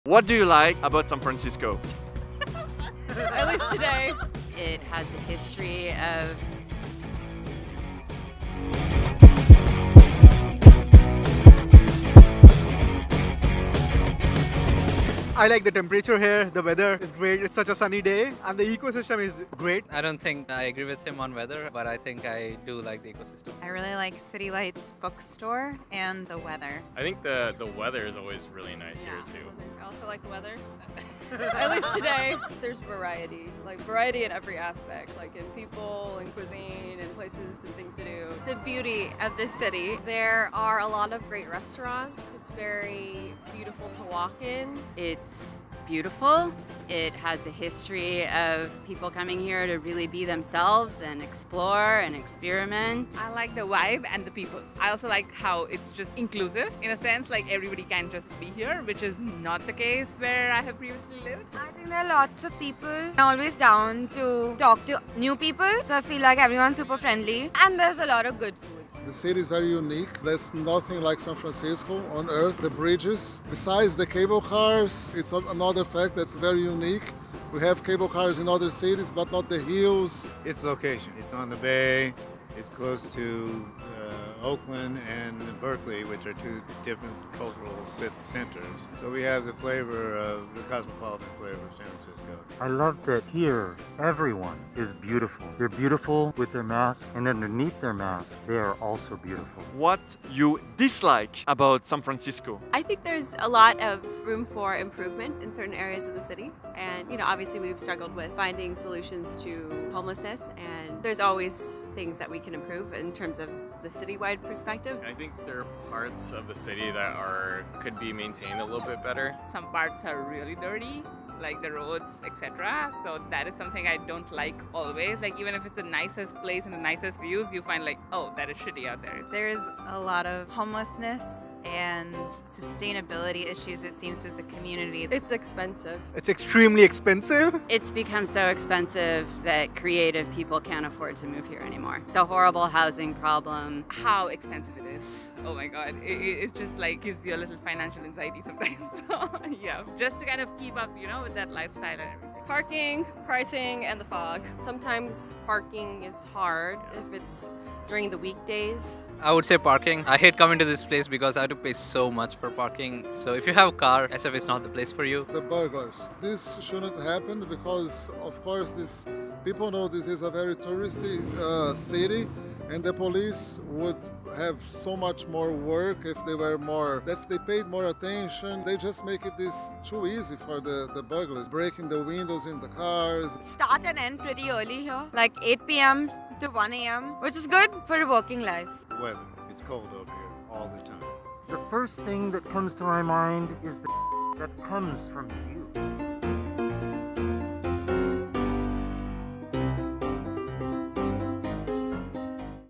What Do You Like About San Francisco? | Street Interviews
San Francisco is a city famous for its stunning views, diverse culture, and iconic landmarks like the Golden Gate Bridge. But beyond the postcards, there's so much more to discover. In this video, I hit the streets of San Francisco to ask locals and tourists alike what they love—or don’t love—about this vibrant city. From the eclectic neighborhoods to the hidden gems, hear firsthand what makes San Francisco a place people either can't get enough of or could do without.